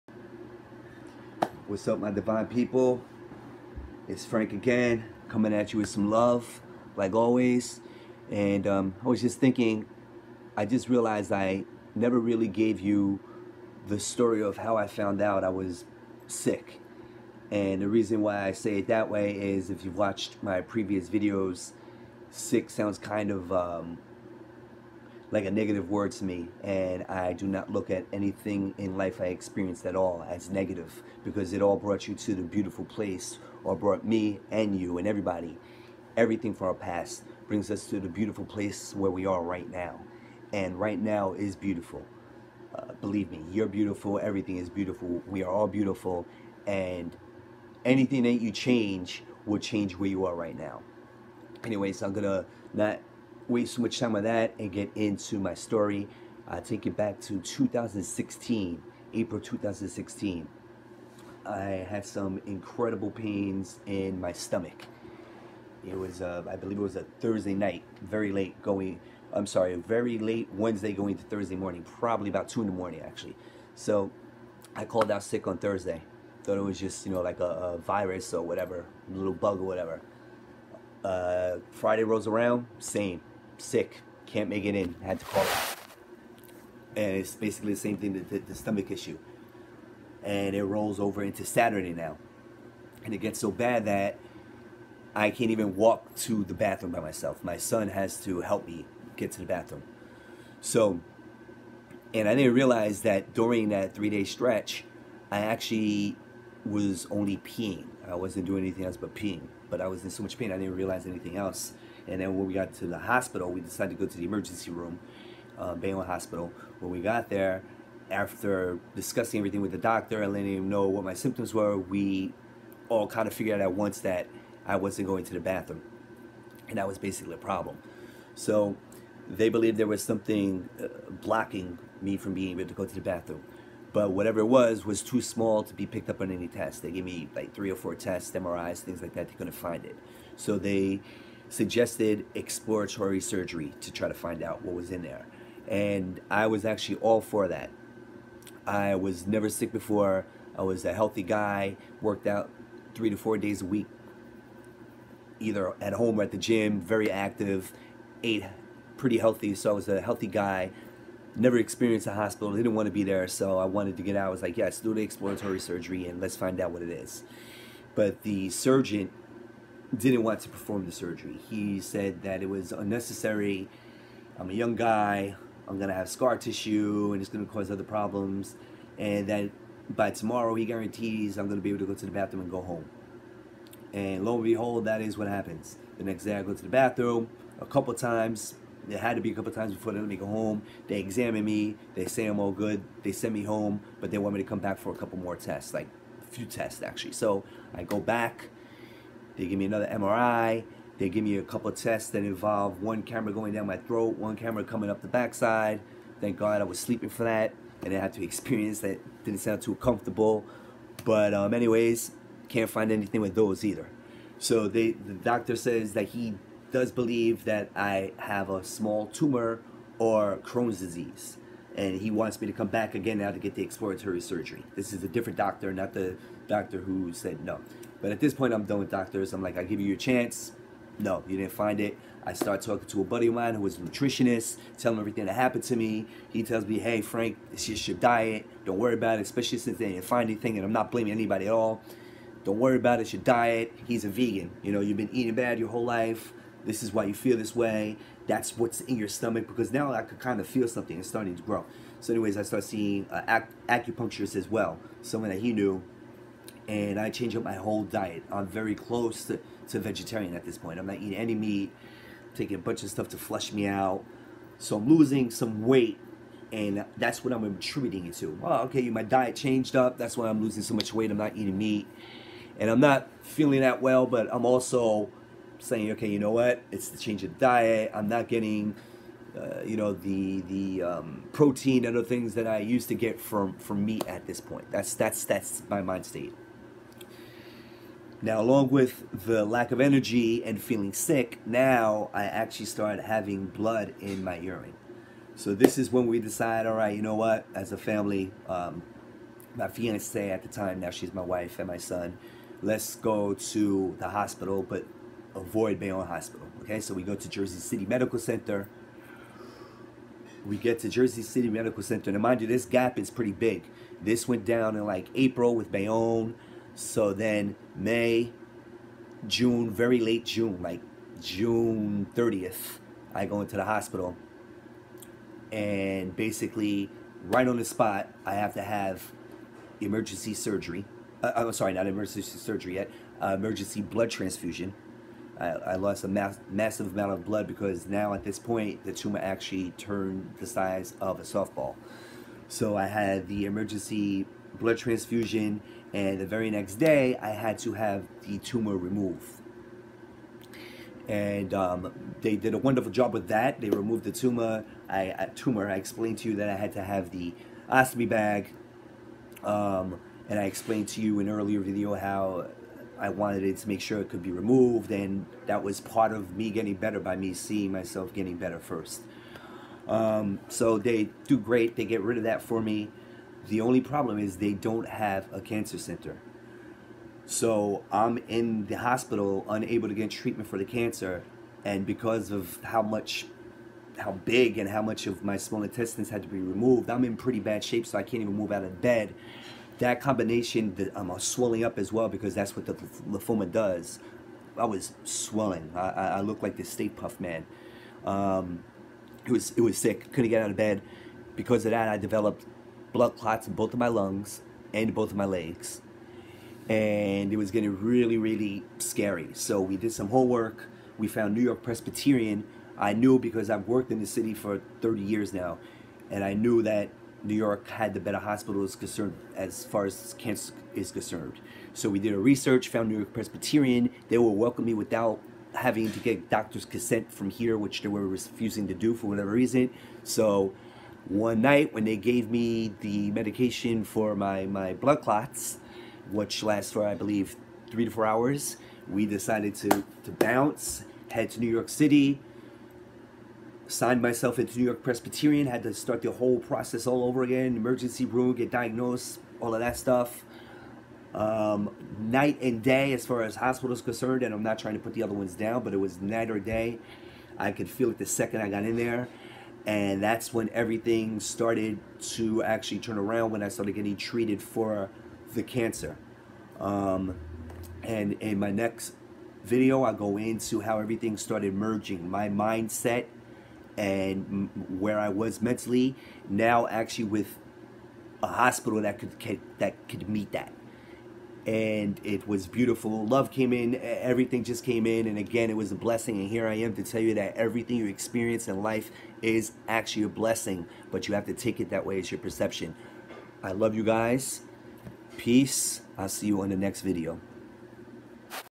Beating Cancer: Part One, my diagnosis
Part one of how I beat cancer and survived Stem Cell